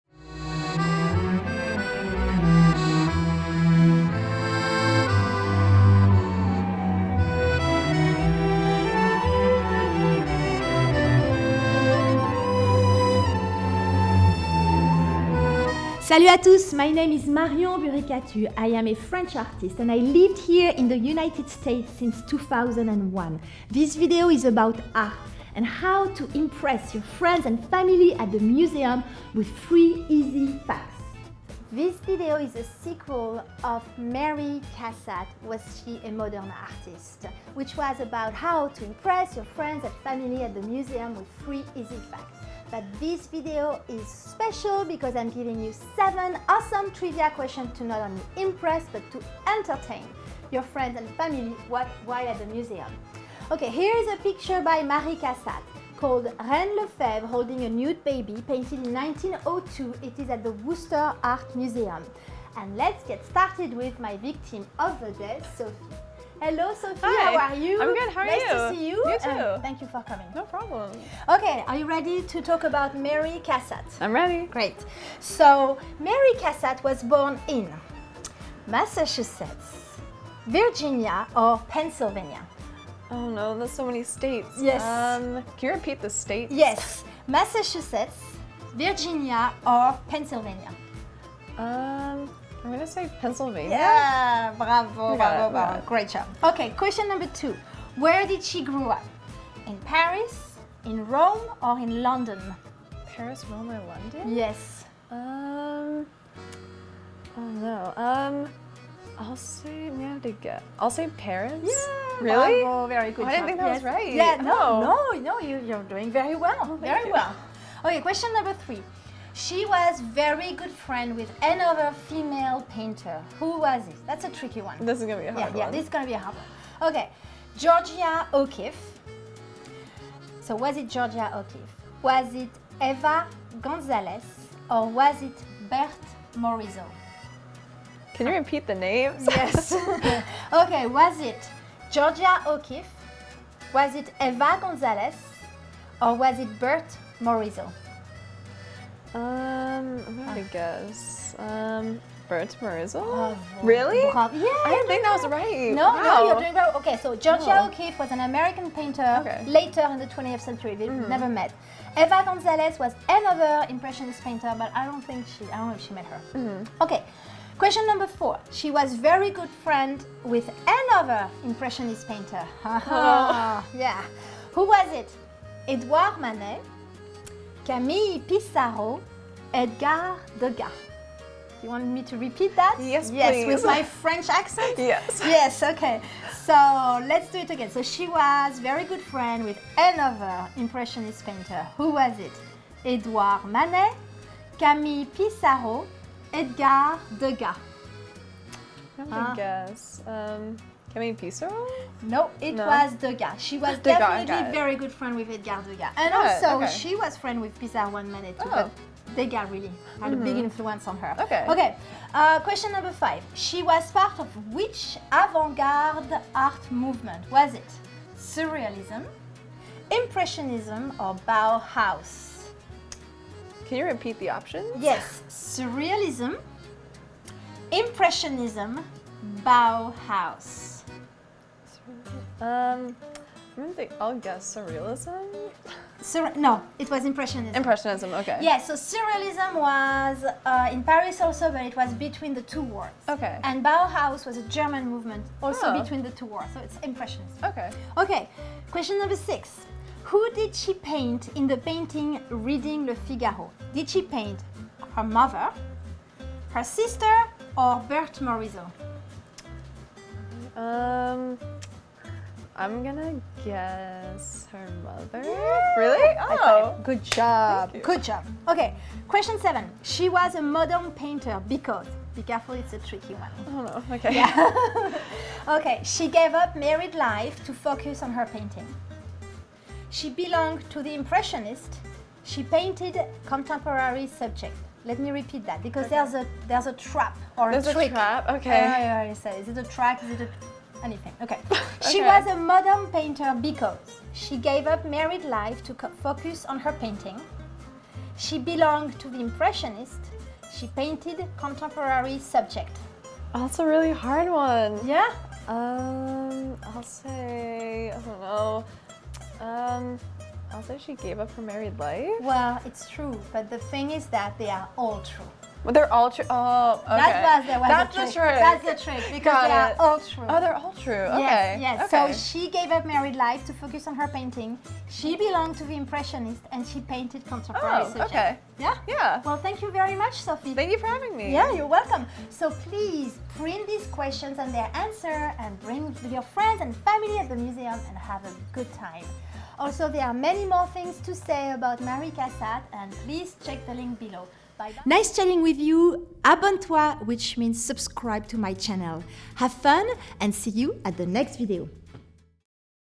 Mary Cassatt | Part I | TRIVIA
ART IN 5'
Hi, I am Marion, a French and American artist. This video is gonna make you an art expert in 5 minutes, just kidding!

This video is the sequel to the video, "Mary Cassatt | Part I." Test your knowledge about Mary Cassatt, and whether you agree or disagree that was a modern woman! Ask your friends and family if they know the answers!

* Women Impressionists: Berthe Morisot, Mary Cassatt, Eva Gonzalès, Marie Bracquemond

Books:
* Berthe Morisot by Dominique Bona
* THE GREATER JOURNEY  - AMERICANS IN PARIS, By David McCullough

Special thanks to the Shrewsbury Media Connection (SMC), who made this video. Check them out here:

TRIVIA QUESTIONS (Correct answers in capital letters)
1) In what state was Mary Cassatt born?
MASSACHUSETTS
Virginia
Pennsylvania

2) Where did Mary Cassatt grow up?
PARIS
Rome
London

3) Which female painter was Mary Cassatt close friends with?
Georgia O'Keeffe
Eva Gonzales
BERTHE MORISOT

4) Which impressionist painter was she close friends with?
Edouard Manet
Camille Pissarro
EDGAR DEGAS

5) She was part of which avant garde art movement?
Surrealism
IMPRESSIONISM
Bauhaus

6) Who did she paint in the painting “Reading le Figaro”?
HER MOTHER
Her sister
Berthe Morisot

7) She was a modern painter because ___?
SHE GAVE UP MARRIED LIFE TO FOCUS ON PAINTING
SHE WAS AN IMPRESSIONIST PAINTER
SHE PAINTED CONTEMPORARY SUBJECTS